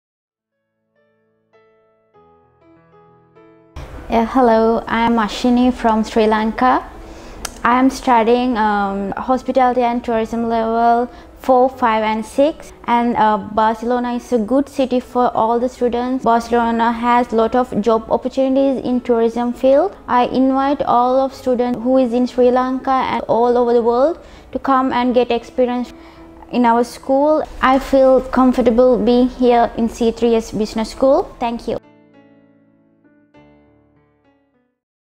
Ashini Illankoon from Sri Lanka studying Tourism & Hospitality Management shares her experience
We asked our students to introduce themselves and to talk about their experience studying with us.

Ashini Illankoon - Bachelor Tourism & Hospitality Management Program . She came to Spain to study, having an international experience, living in different cultures, meeting new people from different countries.

Castelldefels School of Social Sciences (C3S) is a private business school in Barcelona, founded in 2014 and registered under the Spanish Ministry of Education.